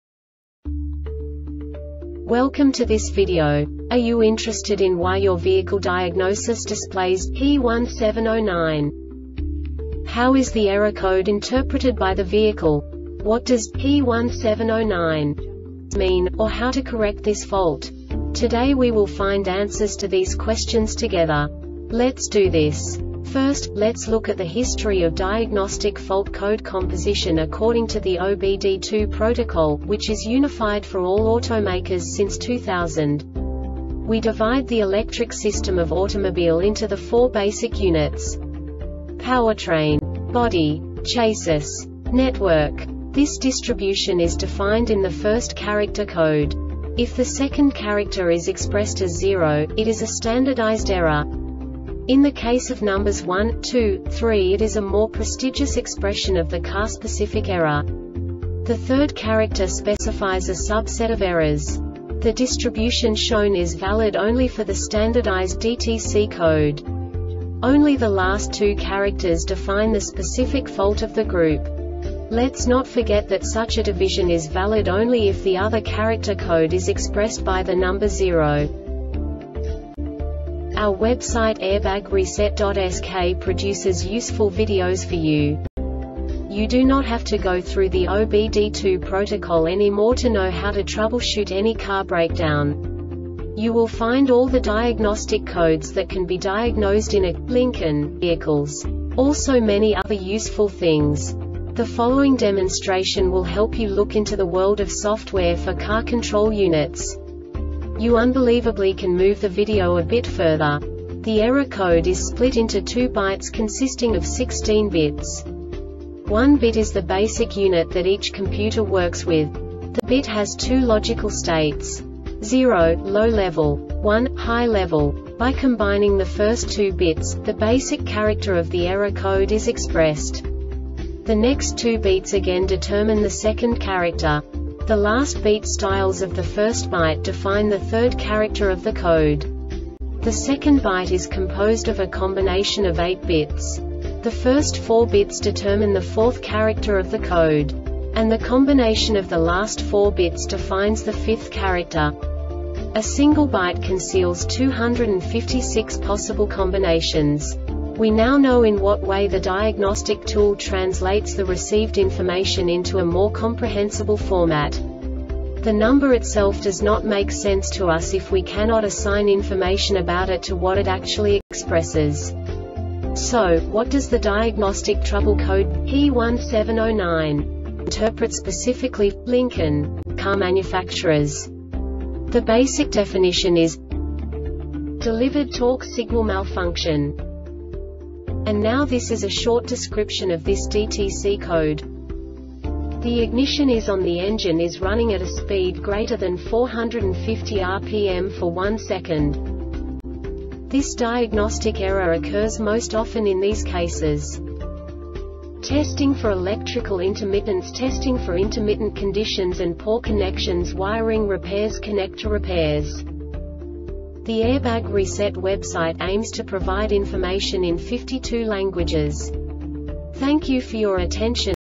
DTC Lincoln P1709 Short Explanation
Contents:
0:21 Basic DTC analysis according to OBD2 protocol standard.
1:48 Insight into programming
2:58 A diagnostically understandable description of the Diagnostic Trouble Code
3:05 Basic error definition

Make:
Lincoln
Code:
P1709
Definition:
Delivered Torque Signal Malfunction
Description: